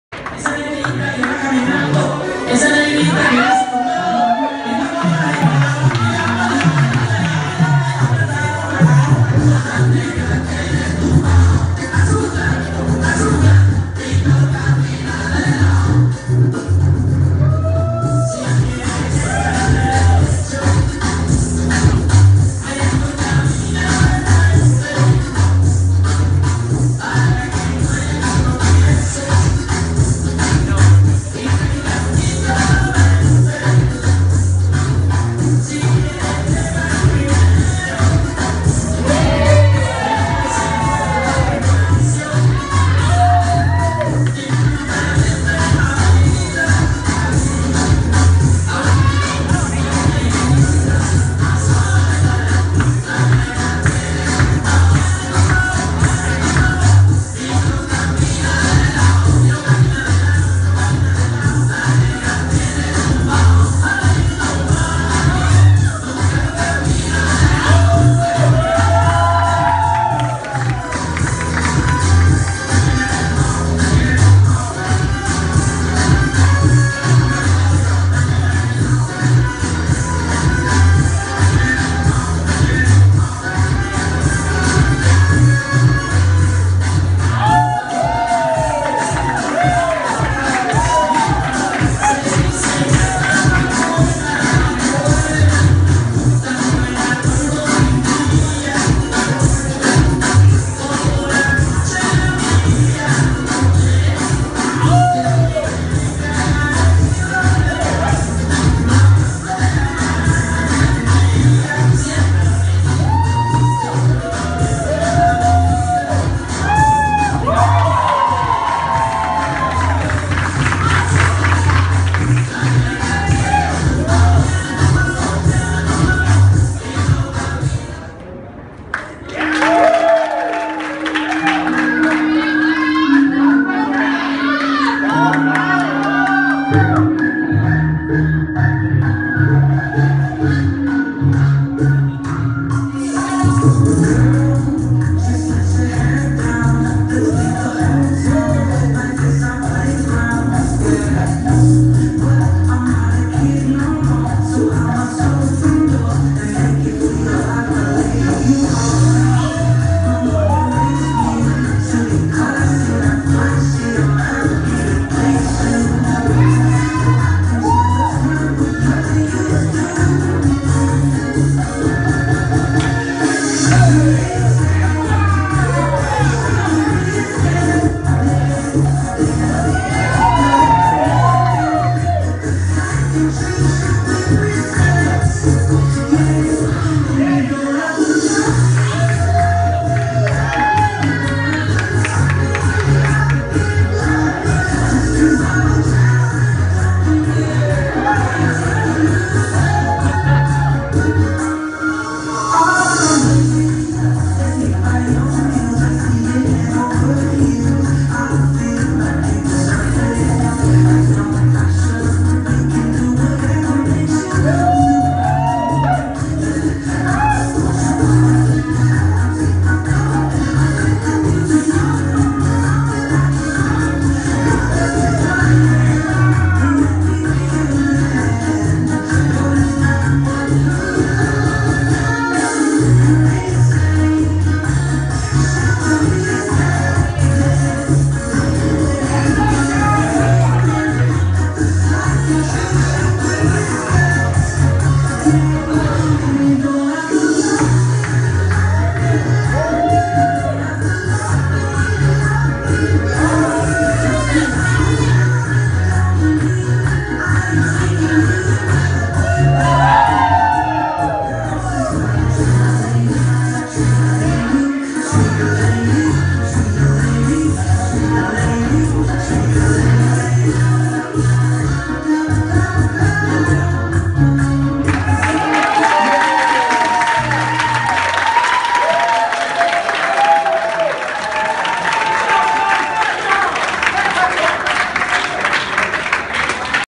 Corazón's 16th Anniversary - Pura Sabrosura
This video features an amazing salsa and bachata routine done by Pura Sabrosura from Pensacola, FL. It was great to have them help us with our weekend events!